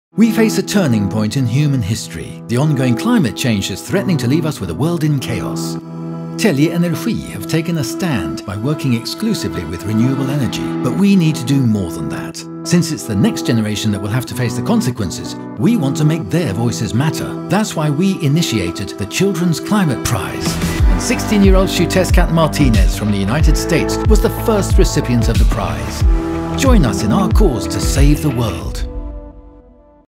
About the Children's Climate Prize
Children's Climate Prize is awarded to a child or youth who has done something extraordinary for the climate and environment. The prize was established in 2016 to highlight another perspective on the climate issue, recognizing that it is the children's future at stake, that their voices are important and deserve to be heard. Telge Energi actively engages in the transition to a sustainable society by exclusively working with renewable energy.
But we need to do more than that.
Telge Energi awarded the first prize in 2016 to the inspiring climate warrior Xiuhtezcatl Martinez, 16, USA. In November it's time to announce the young climate hero of 2017.This year's Winners and Finalists are appointed by a jury and the winner will receive a medal, diploma and a prize fund of SEK 50,000.